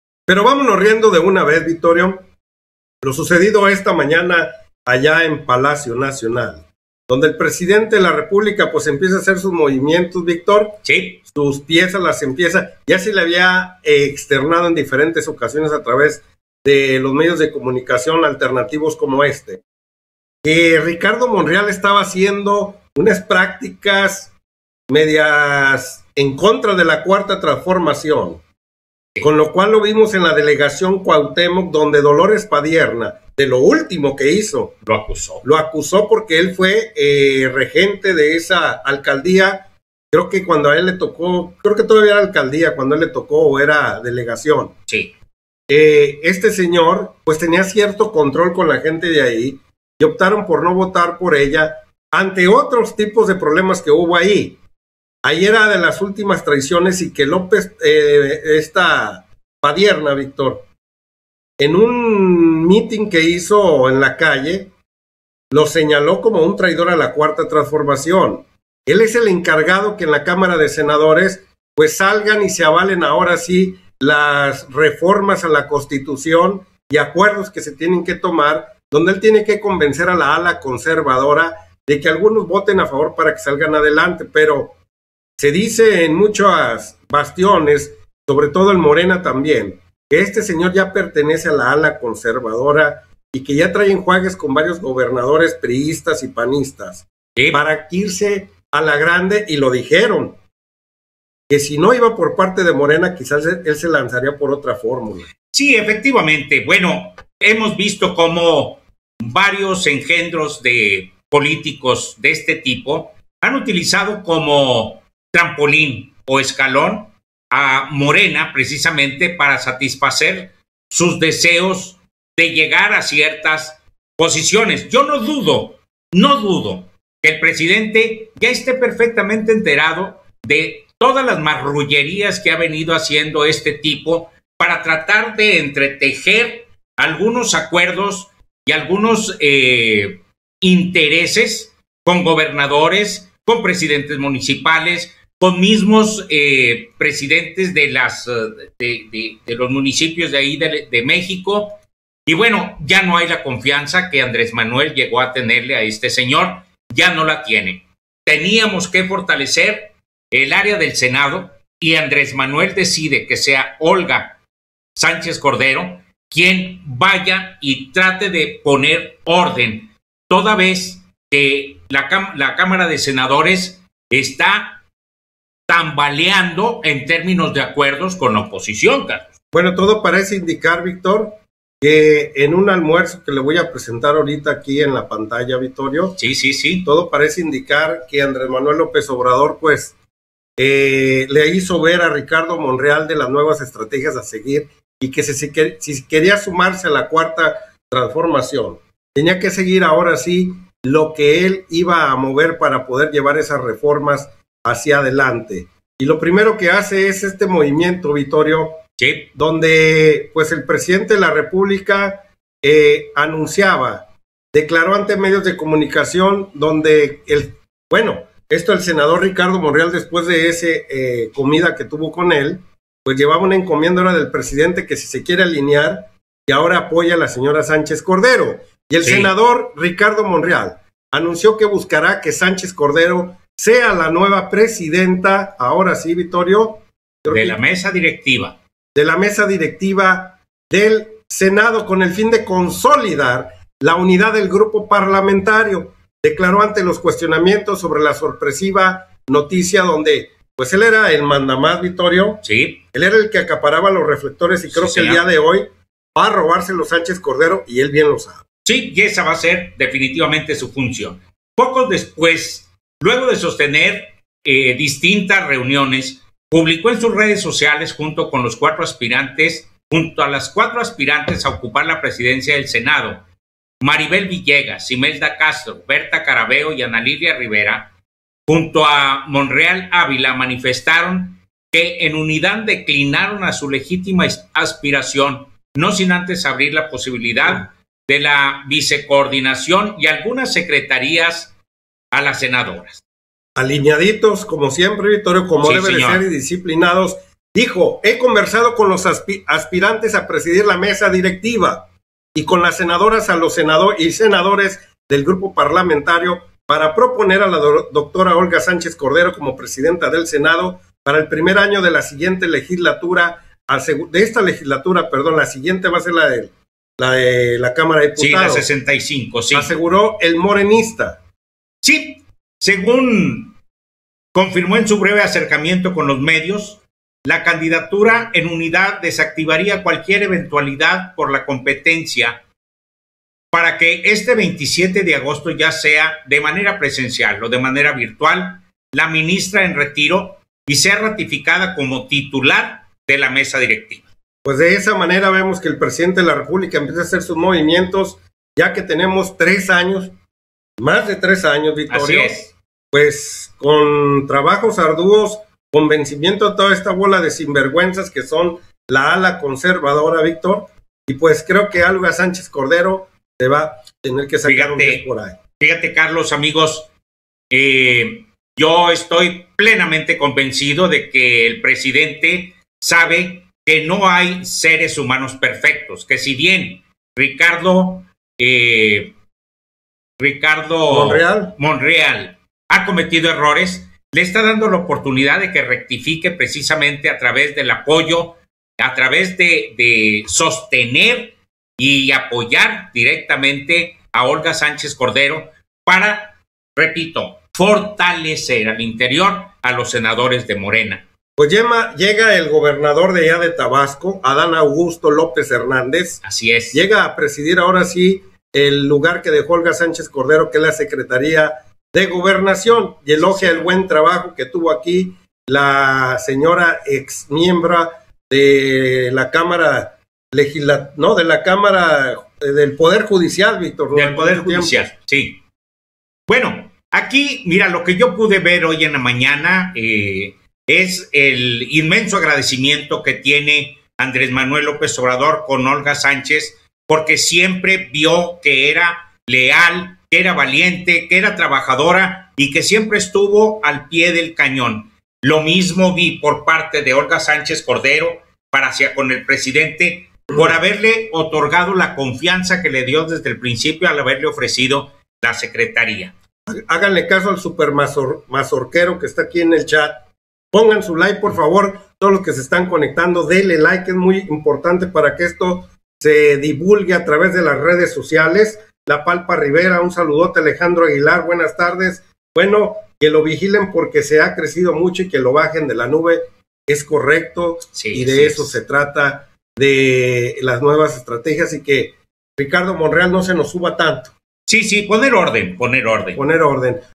Dra Olga Sánchez Cordero deja la Segob y desbanca a Ricardo Monreal en el senado
Fueron muchas las acciones de deslealtad de Ricardo Monreal y AMLO se harto de ver que llevaba una dirección diferente a Morena, le dá la última oportunidad

APORTACIONES VOLUNTARIAS AL CANAL:
SANTANDER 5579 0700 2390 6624